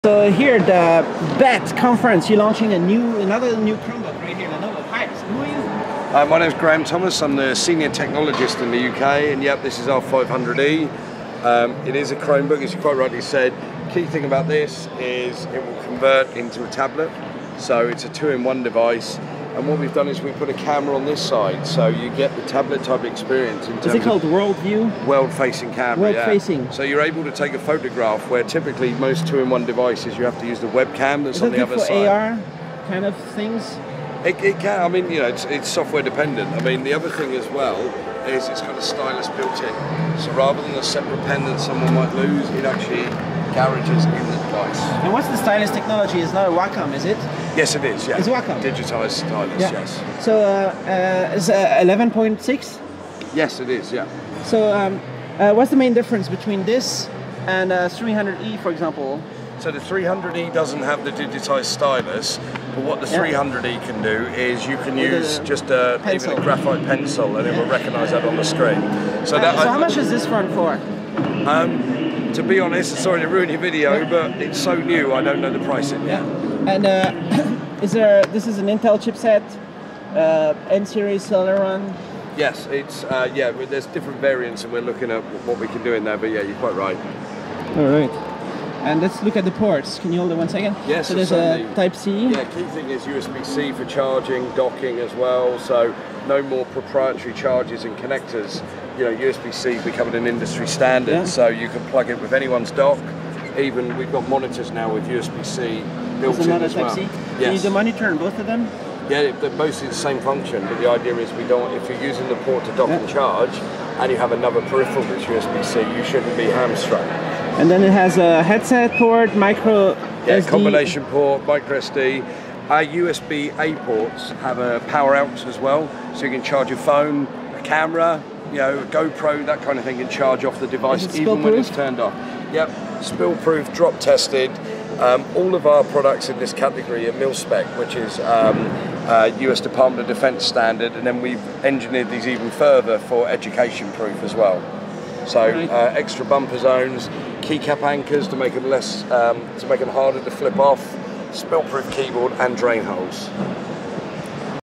Lenovo 500e Chromebook
Lenovo 500e Chromebook is a 11.6” 2-in-1 Chromebook with a 360-degree hinge for four modes of interaction, a garaged EMR (electromagnetic resonance) pen, features like Two HD cameras, 720p front-facing camera and a 5MP world-facing camera.